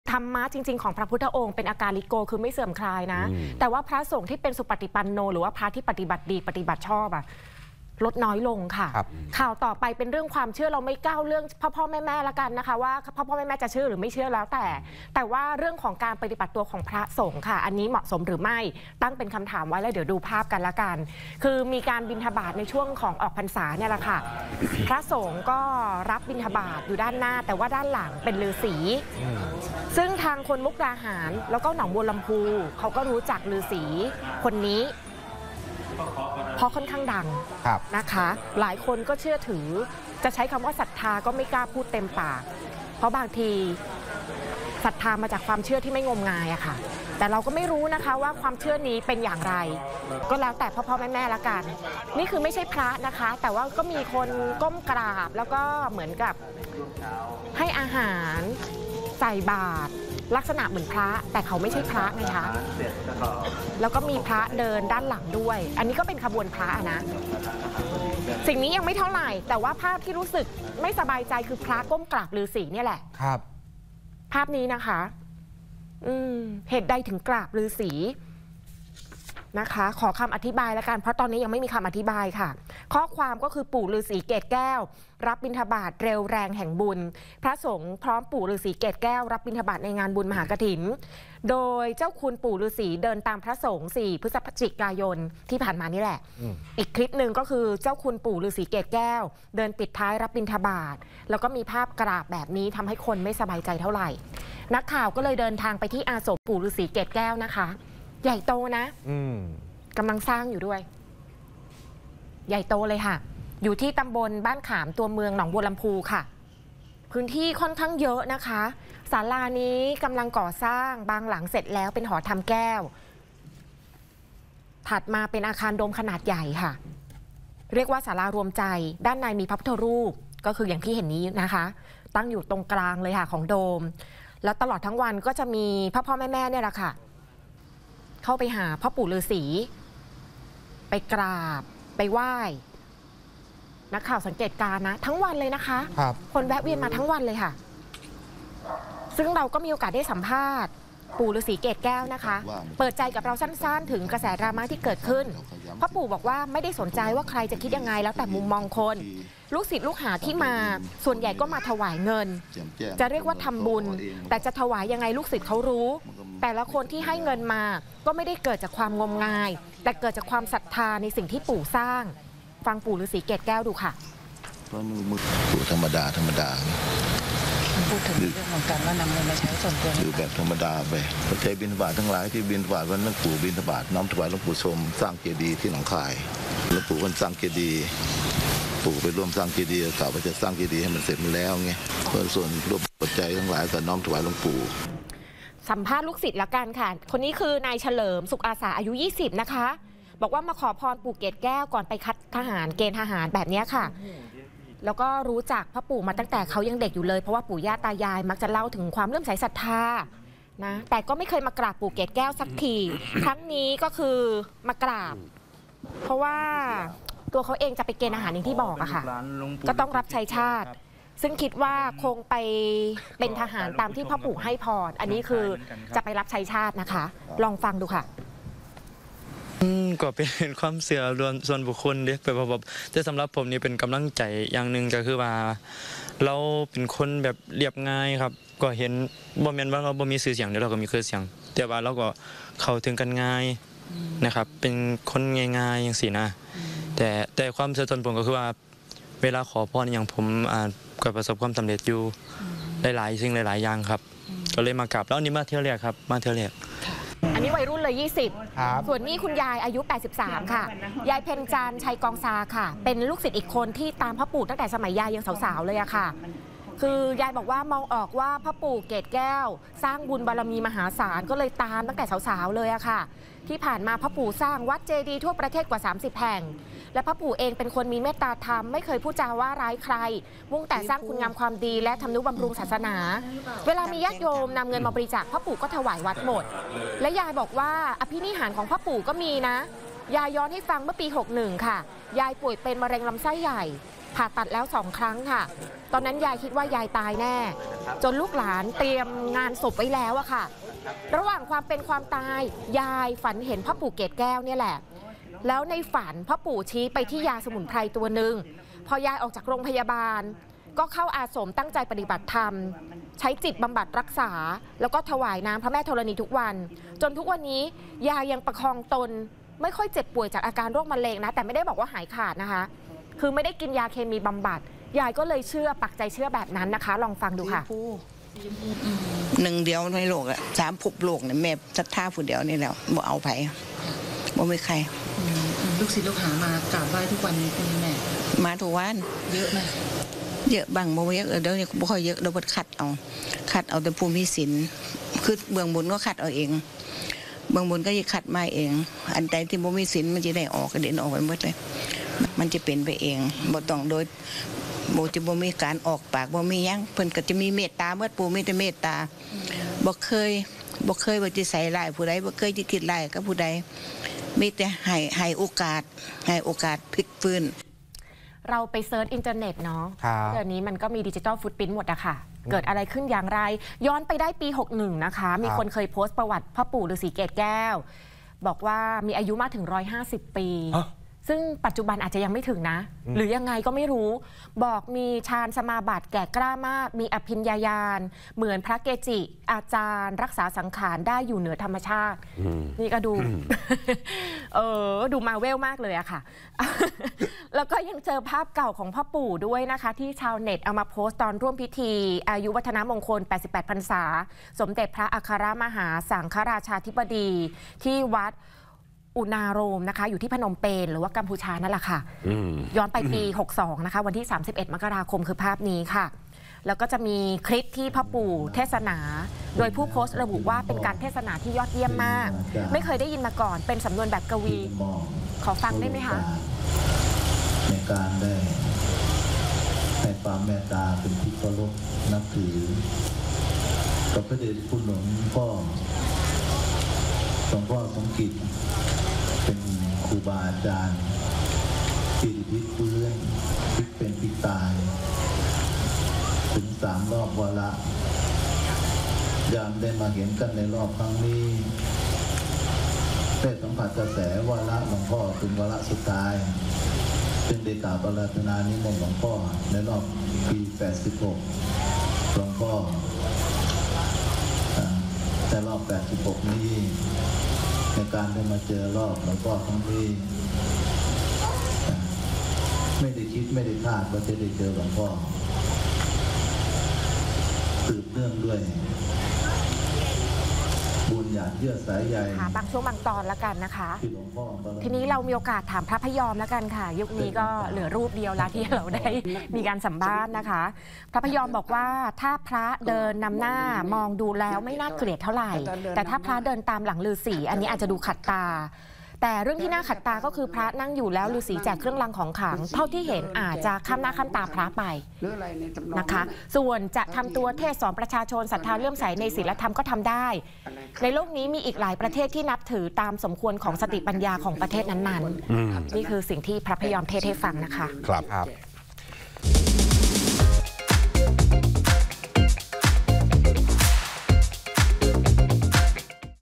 ดรามาปู่ฤาษีเกศแก้วให้พรพระสงฆ์ | ข่าวช่อง8
จากที่มีกระแสโจมตีกรณี มีพระสงฆ์ก้มกราบชายที่อ้างตนว่าเป็นปู่ฤาษี อีกทั้งยังมีคลิปที่ฤาษี ออกรับบินฑบาตในงานบุญมหากฐิน จึงเกิดคำถามตามมาว่าเหมาะสมหรือไม่